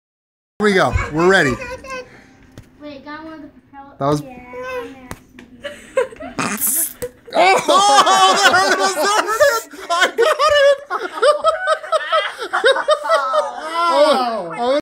Buff arm/bicep punch
If your buff and you punch your own arm... here is what happens